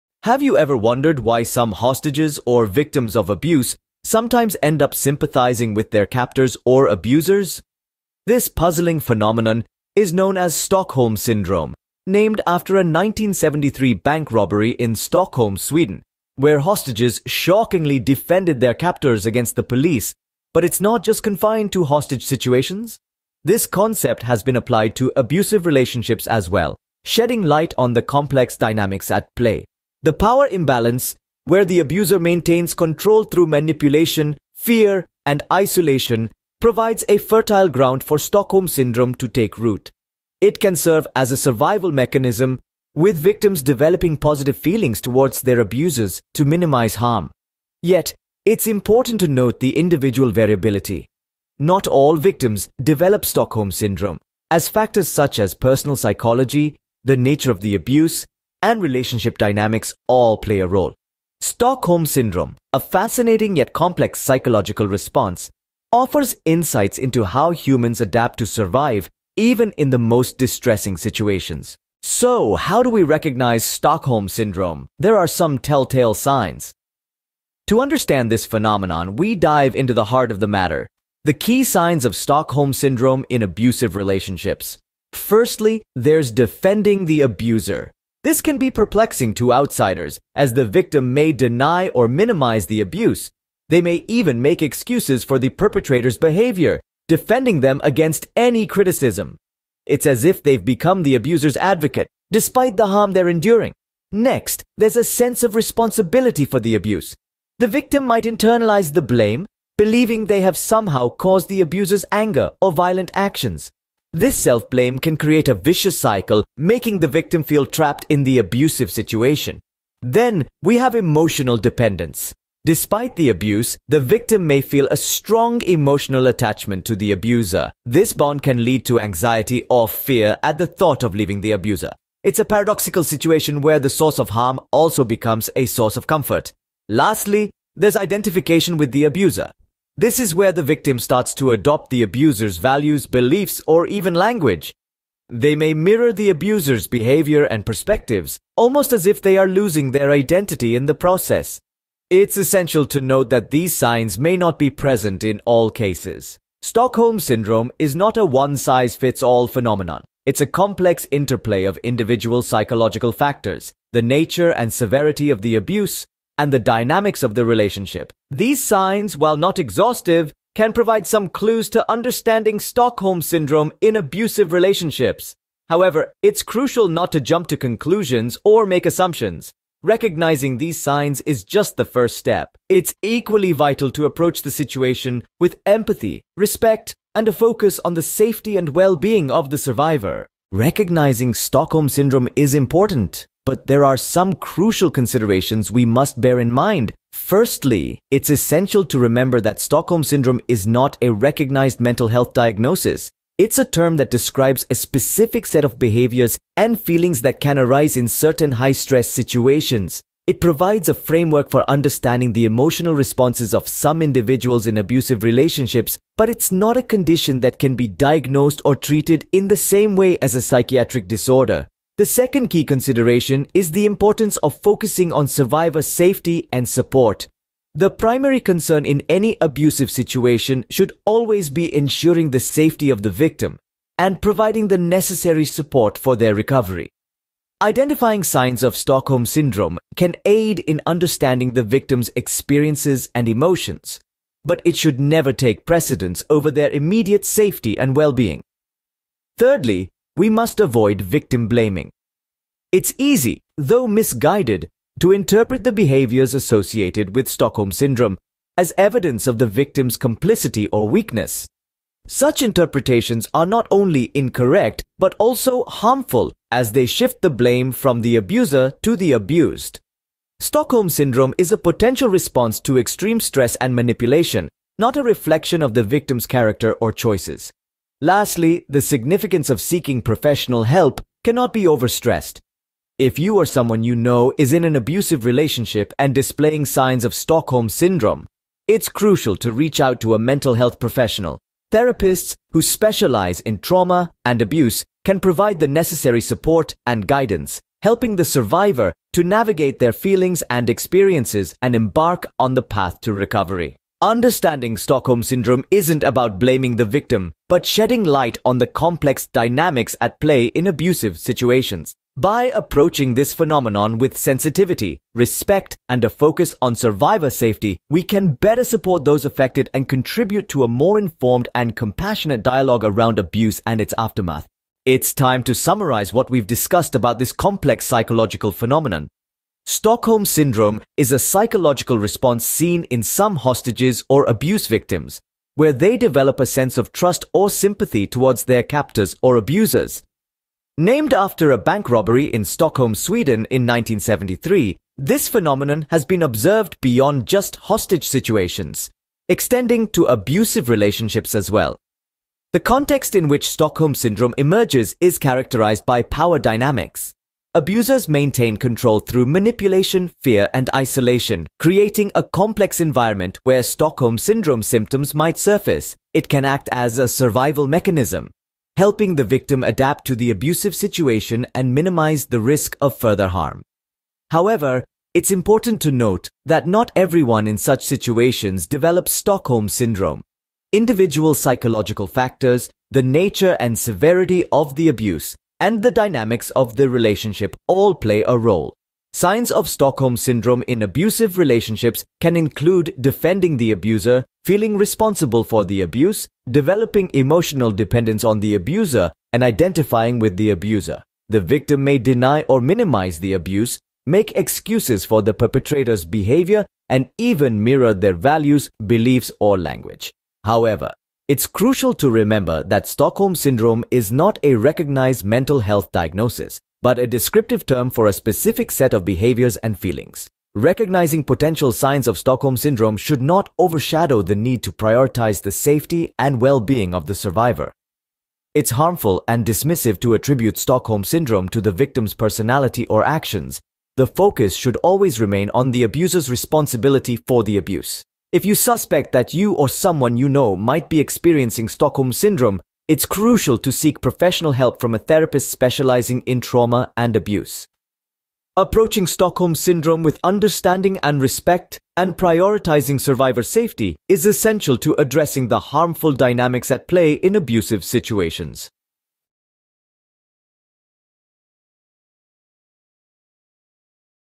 The Stockholm Syndrome
In this thought-provoking video, we delve deep into the intricate world of Stockholm Syndrome. Join us as we unravel the origins, manifestations, and profound implications of this complex psychological phenomenon in abusive relationships.

With expert insights and real-life examples, we shed light on the psychological processes that lead individuals to develop an empathetic bond with their abusers. Understand the intricate dynamics at play and gain a deeper understanding of the emotional and mental complexities within these toxic relationships.

Through our exploration, we strive to foster awareness and promote empathy for those affected by Stockholm Syndrome. Whether you're a psychology enthusiast, curious about human behavior, or looking to support those in need, this video is an essential watch.

Help us make a difference by sharing this video with your friends and family. Together, let's spark important conversations and create a more compassionate world.

OUTLINE:

00:00:00 The Intriguing Phenomenon of Stockholm Syndrome
00:01:11 Unraveling the Signs of Stockholm Syndrome
00:03:10 Important Considerations about Stockholm Syndrome
00:05:24 Remembering the Key Points